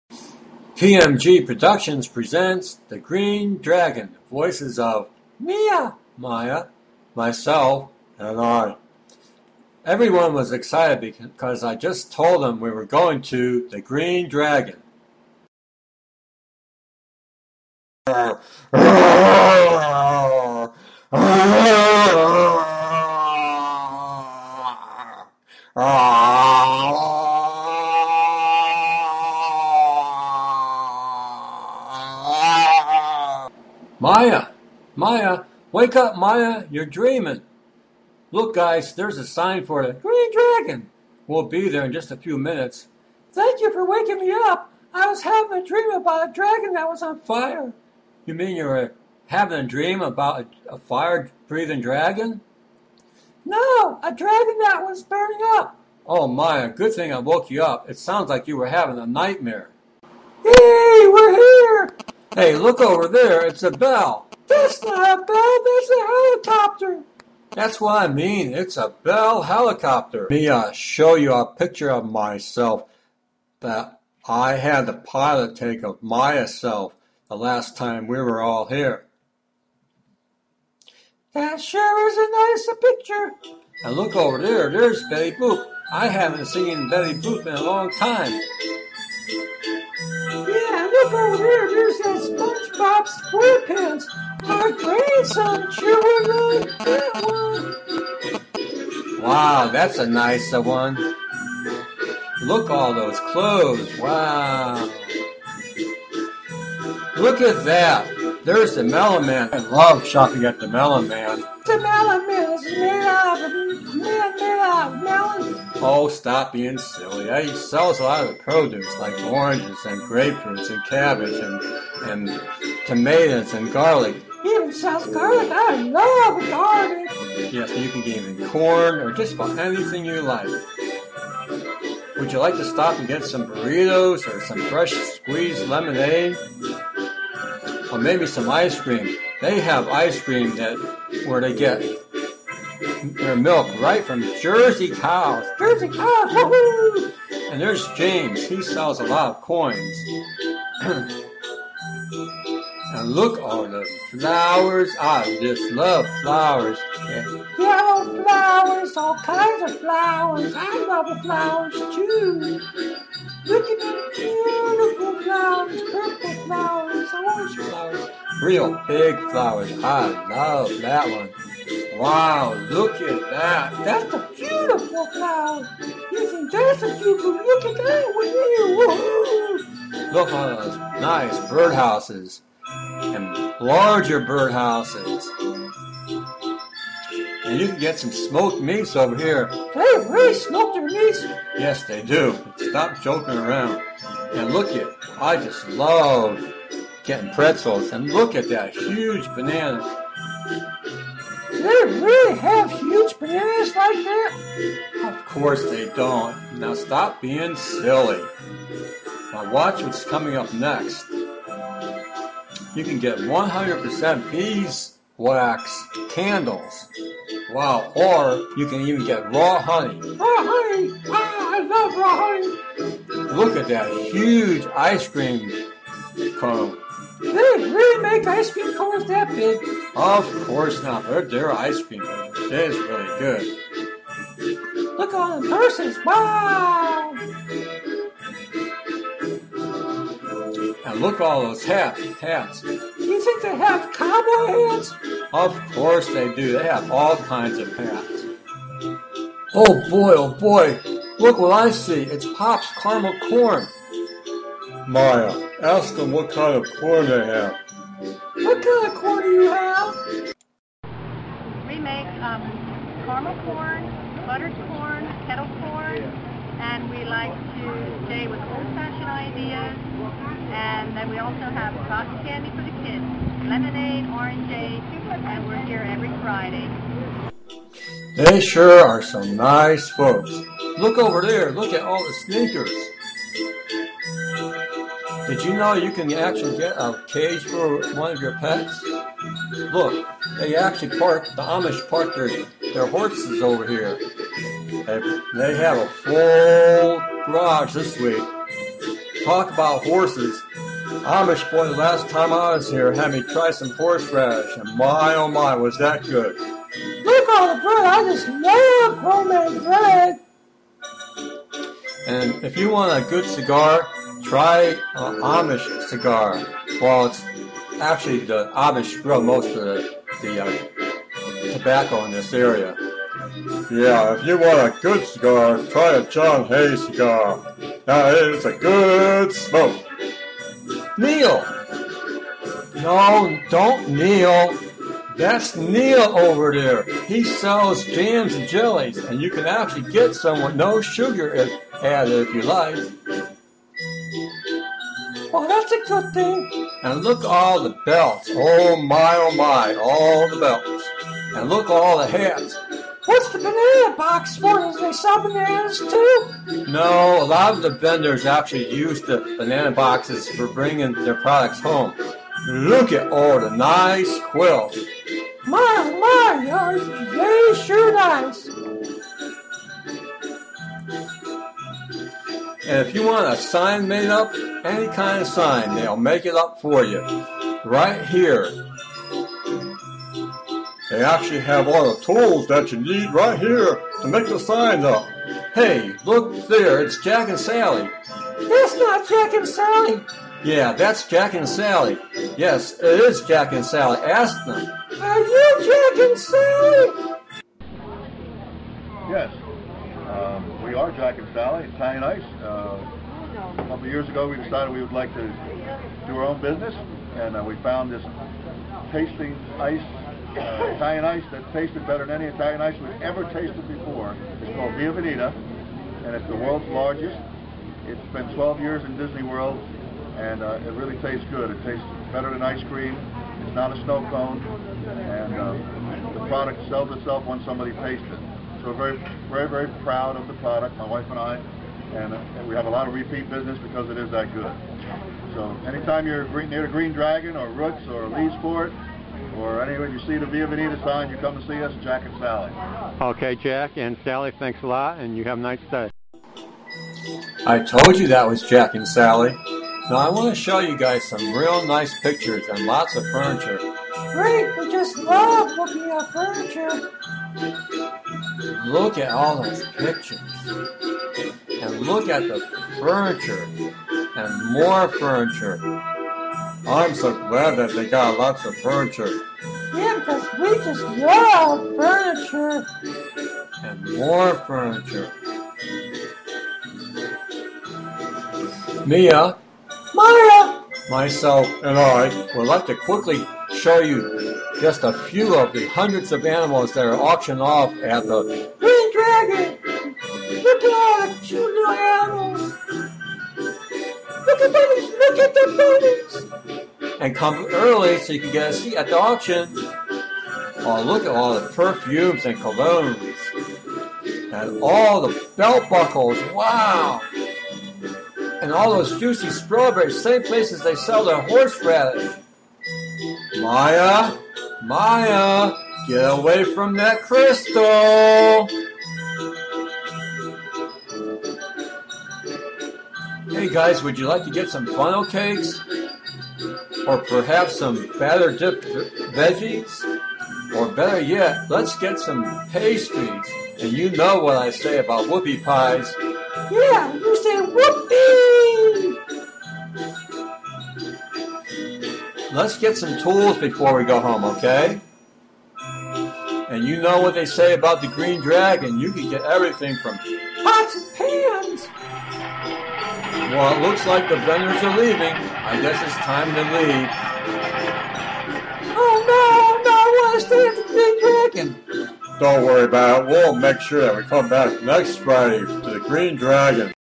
Flea Market! The Green Dragon ( FUNNY VOICE OVER )
I am definitely not a Don LaFontaine! or a David Kaye, but I hope you enjoy my sense of humor about,The Green Dragon! A Real large Flea market in Pa.  The Green Dragon is open every friday. If you liked my sense of humor, you might want to watch, Pine Cradle Lake, and my newest voice over The 2011 Philadelphia Flower Show were I run into John Wayne, Arnold Schwarzenegger and Julia Childs!